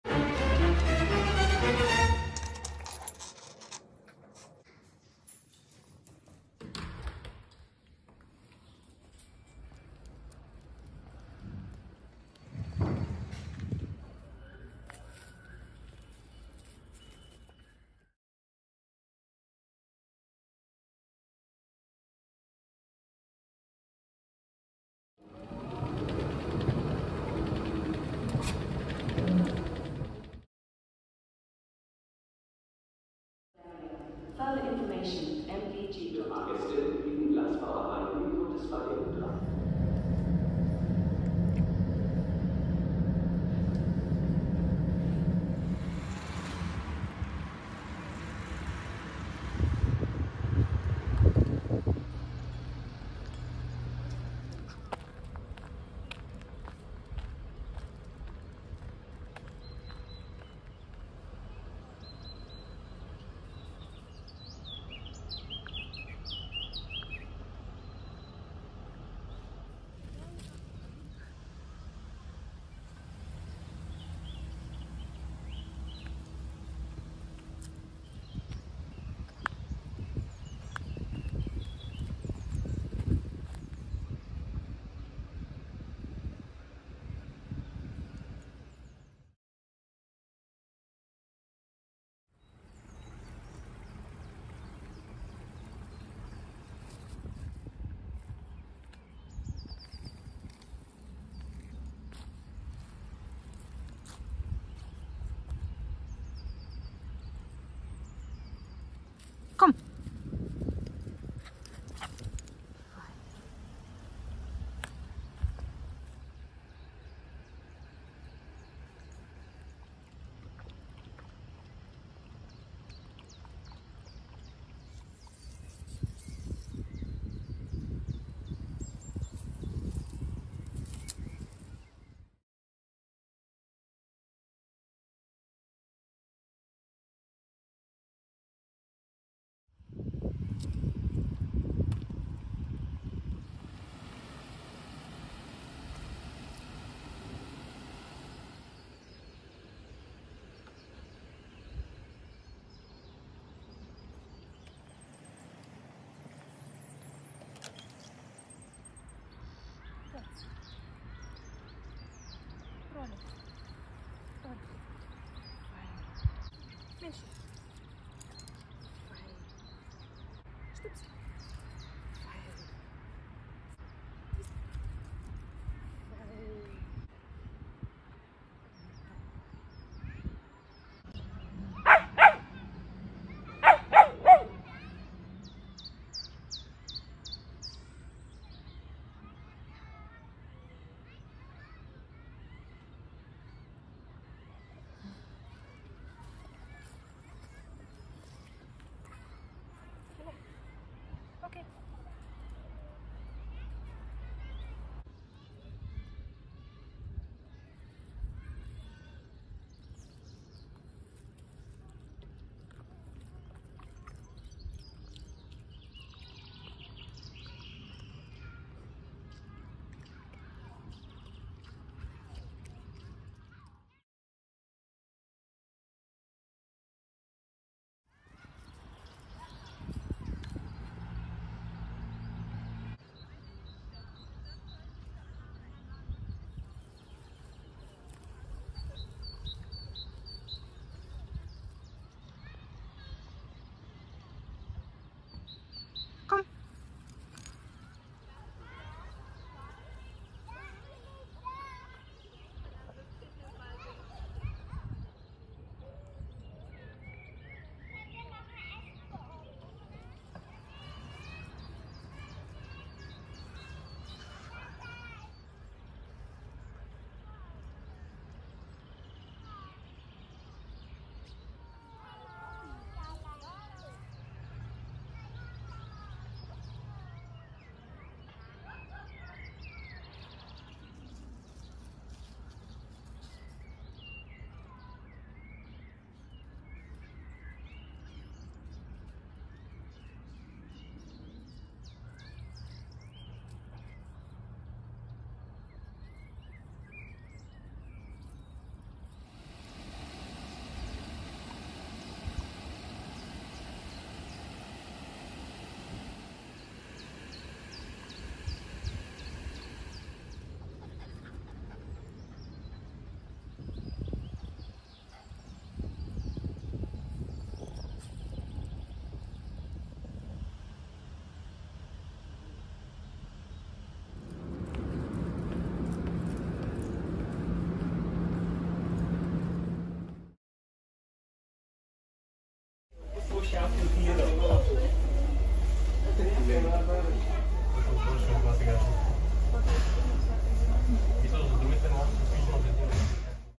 Cavalier King Charles Spaniel. Where to walk the dog in Munich?
Today it's a very nice weather and I decided to give unusual walk to my dog. We went to Garching-Forschungszentrum. You will see at this video how nice the area is. It's like a big park with not many people and dogs. If you live in Munich , you know that it is very difficult to find not a crowded place.